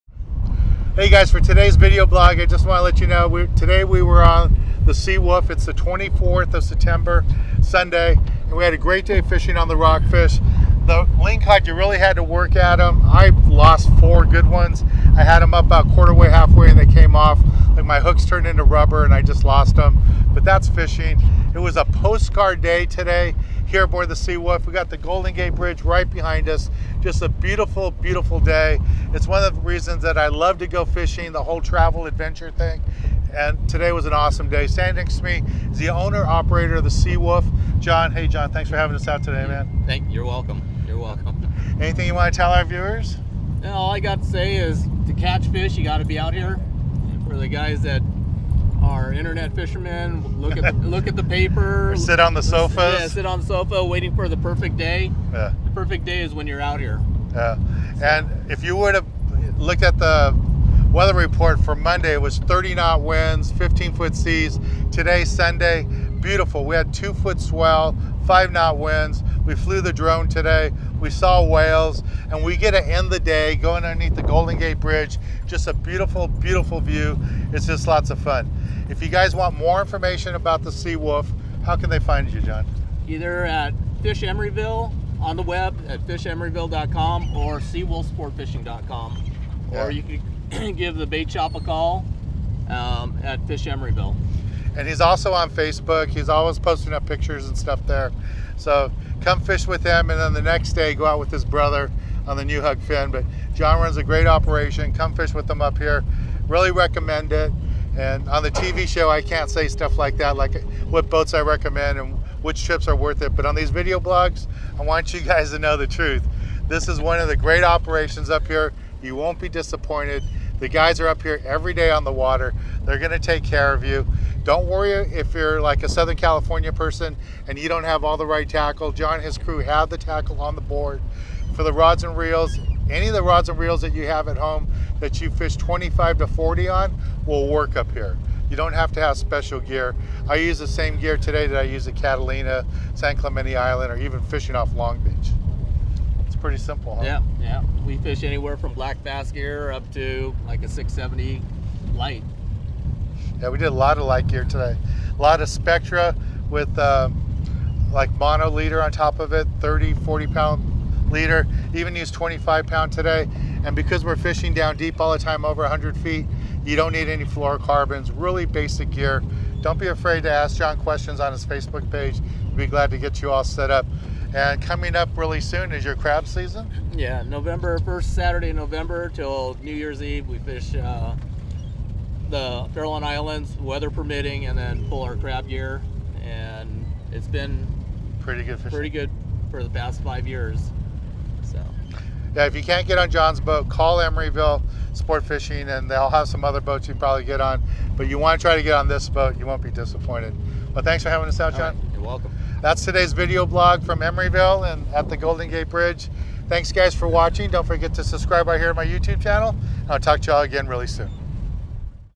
Sea Wolf Fishing Re- Cap Report | SPORT FISHING
Dan talks with the capt. of the Sea Wolf about how the day of fishing event and what they have planned for the rest of the season.